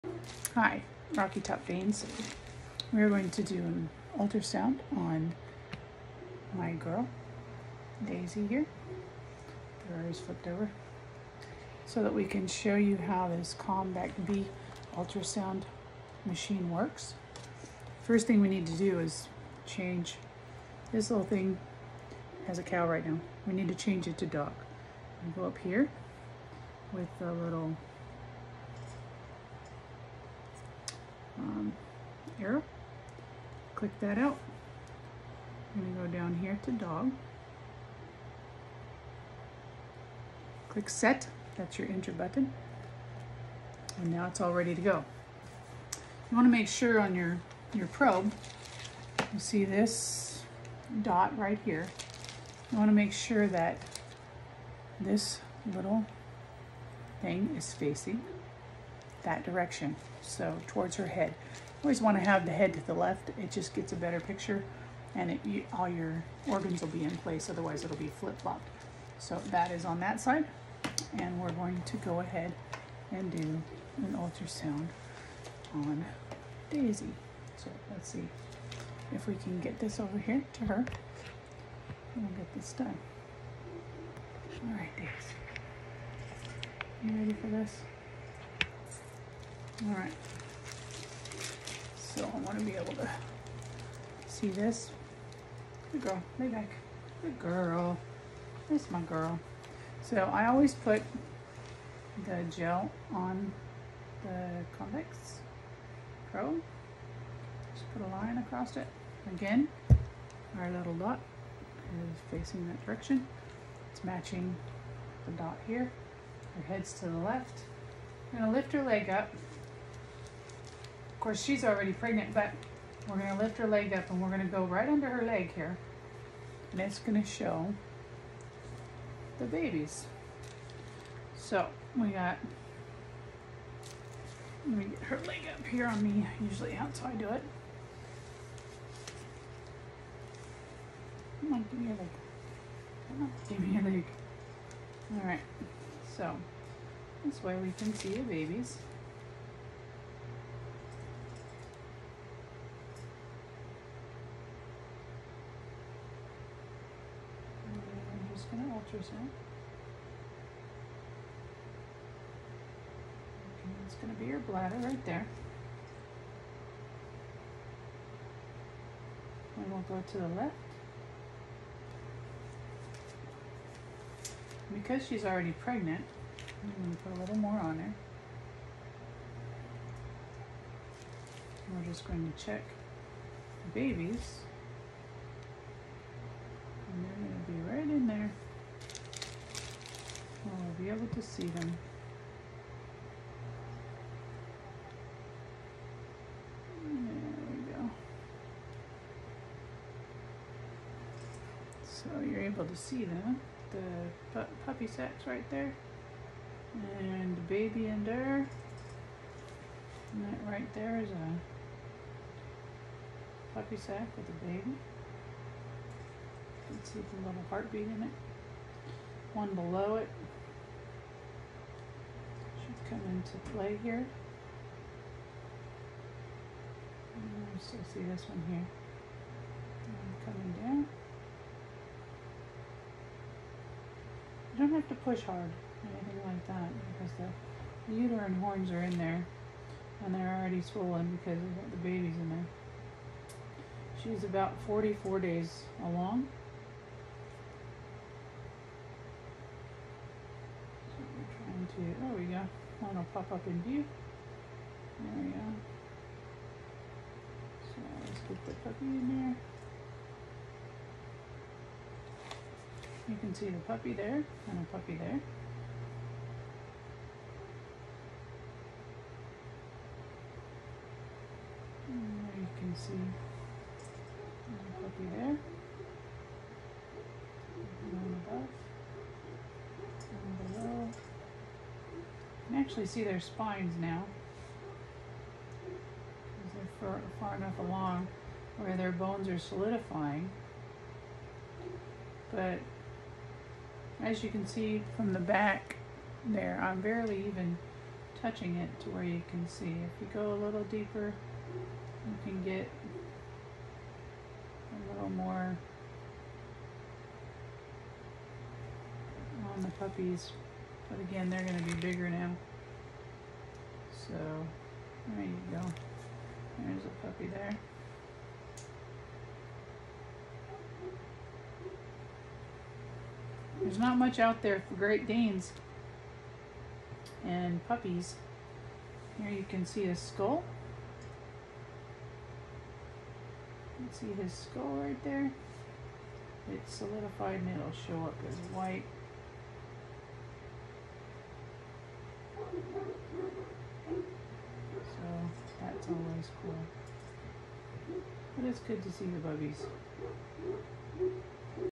Ultrasound Great Dane at 44 days. How to on the Contec B ultrasound machine & 3.5 mhz convex probe
RockyTop Danes ultrasound demo of using the Convec B ultrasound machine with a 3.5 mhz convex probe to see the puppy sacs and puppies at 44 days gestation. Disclaimer: (I am not a vet or vet tech) I have been trained on the ultrasound machine and what to show and how to use it to check on puppies maturing in the Dam. @RockyTopDanes